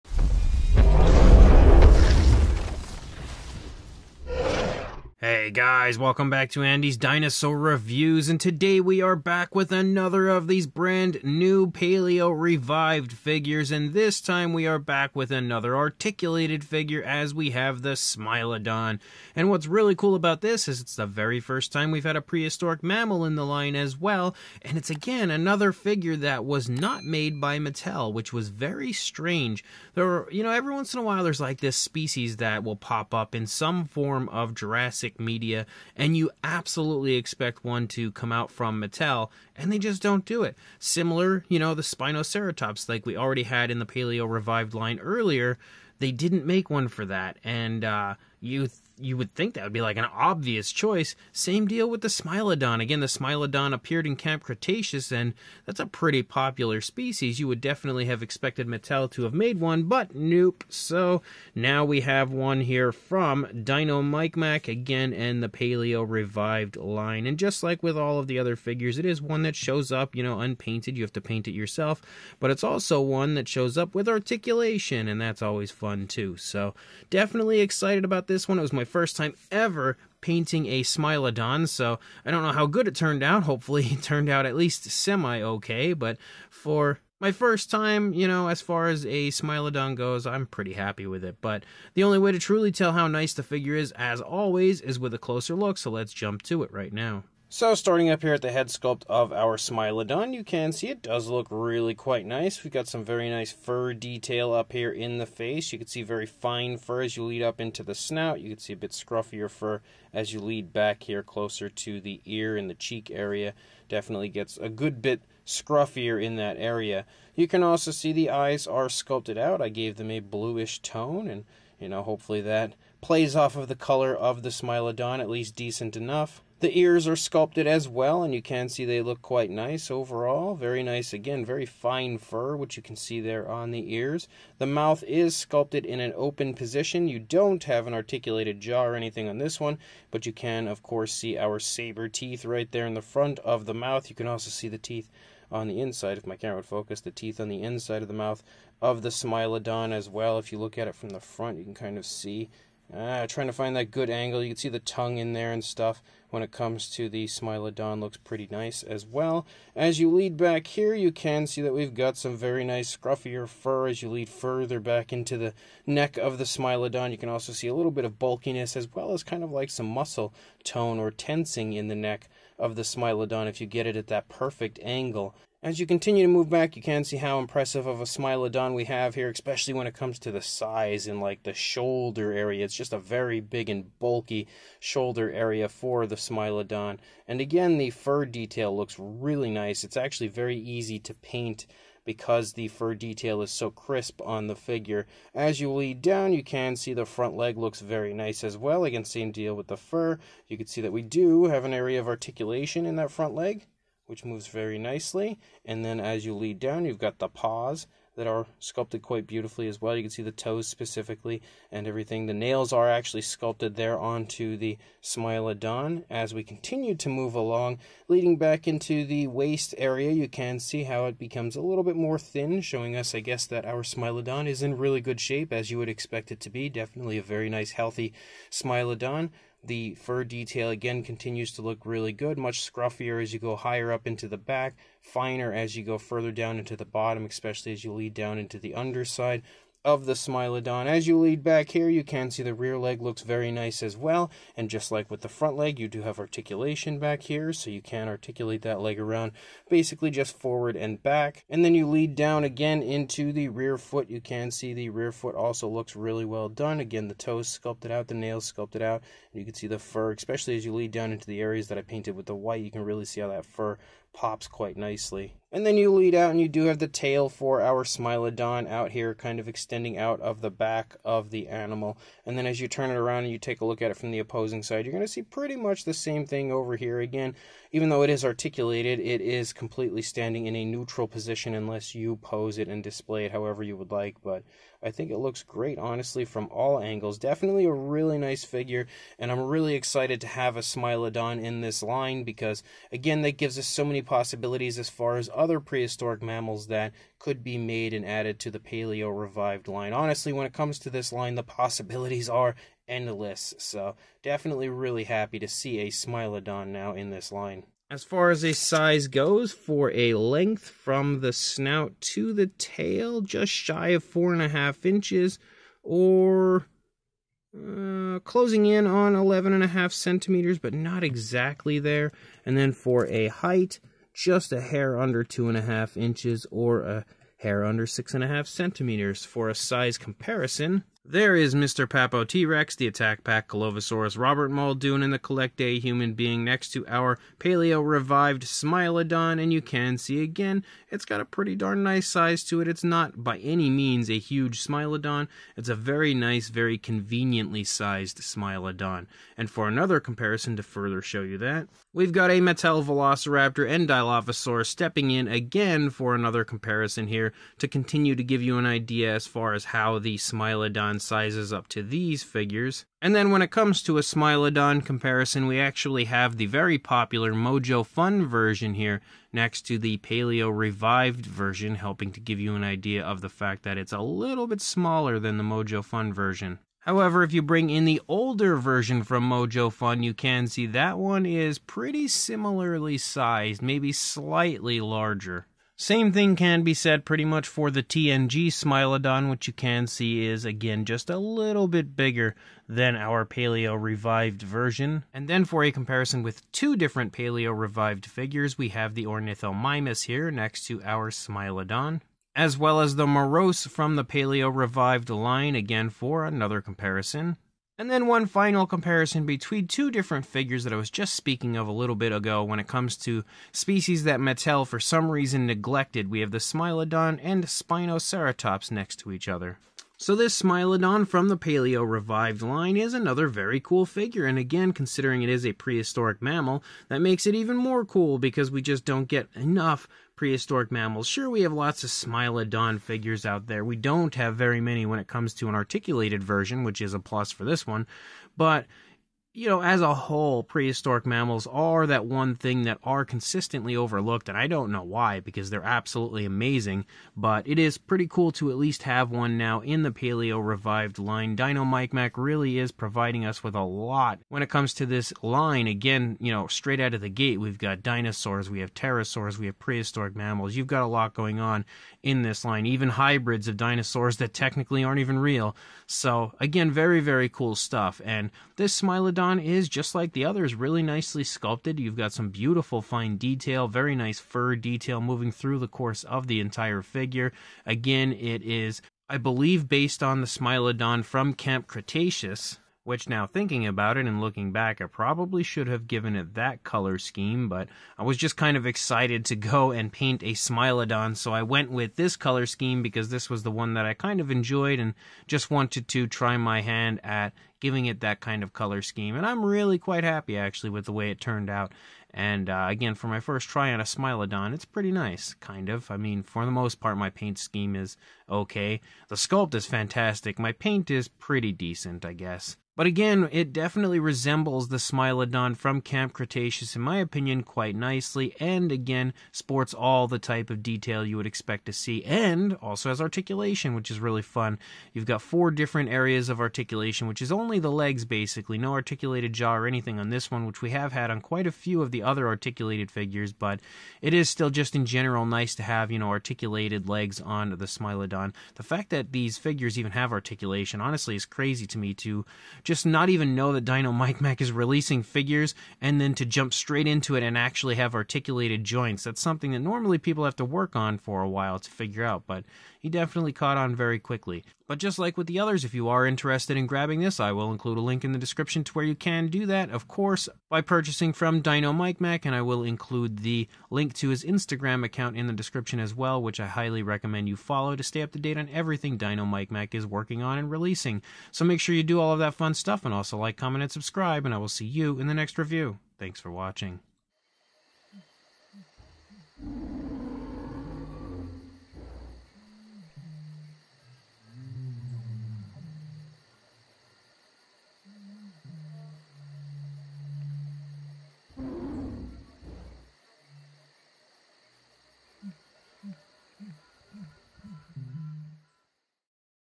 Paleo Revived articulated Smilodon Review!!!
Today we're checking out yet another brand new Paleo Revived release as this time we've got an articulated Smilodon!!!

Would you like to send something to have reviewed, unboxed, or promoted on Andy's Dinosaur Reviews??? Have some requests???